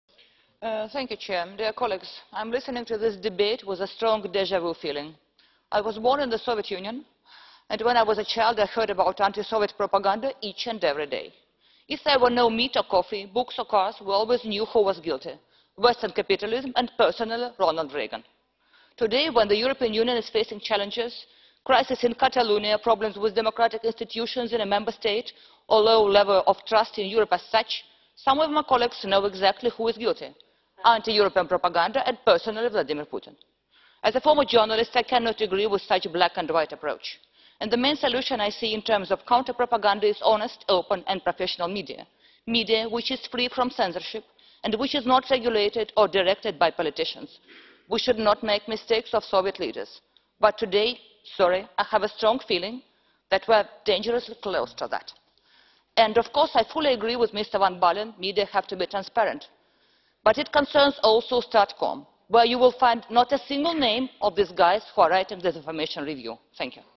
Yana Toom 17 Jan 2018 plenary speech on Russia
Yana Toom 17 Jan 2018 plenary speech on Russia - the influence of propaganda on EU countries